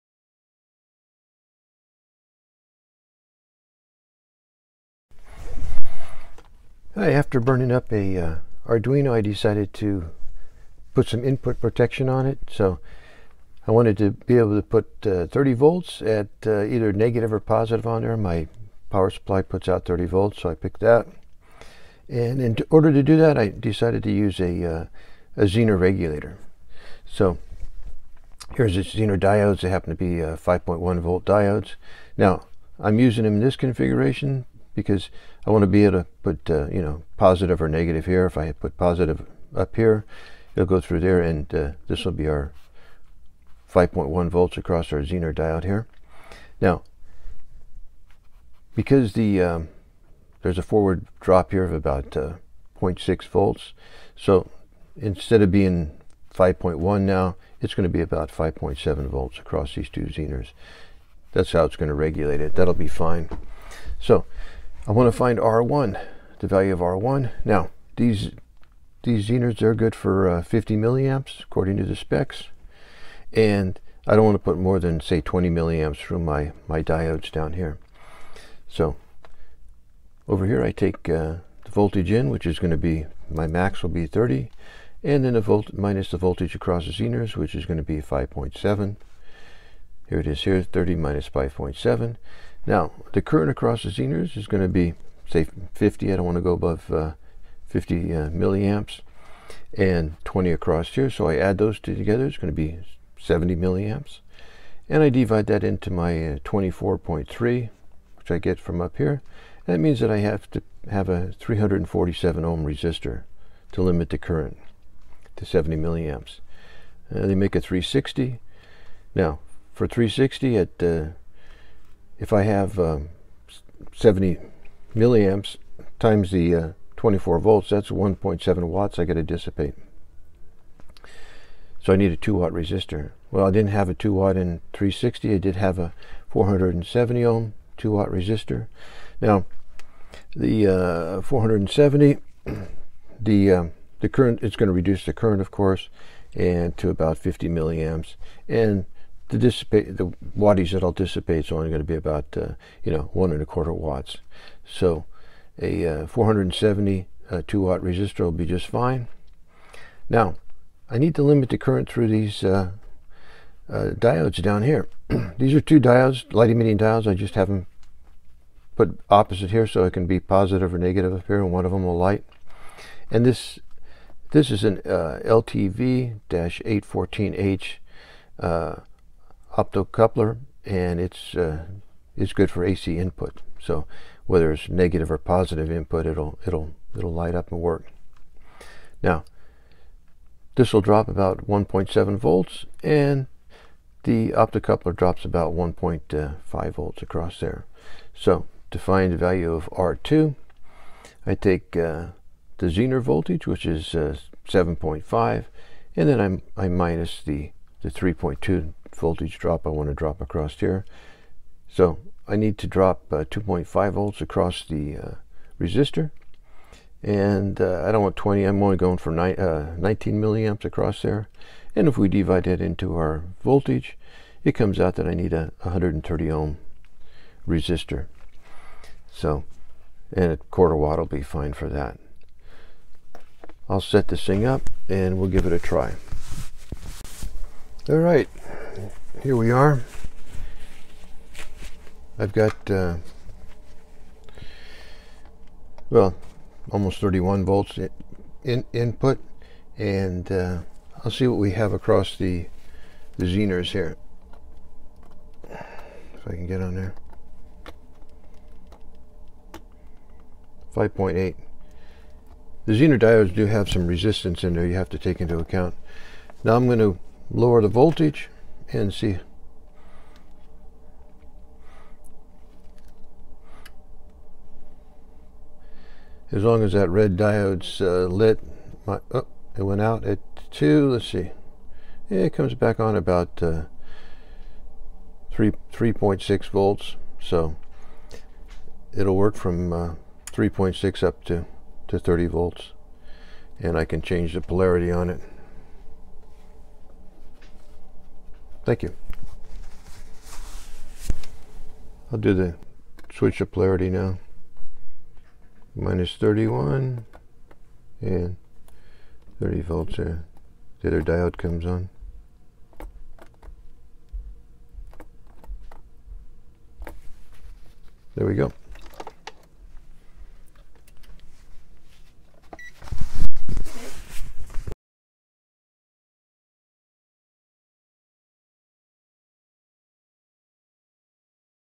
Arduino input protection
Using a zener regulator for input protection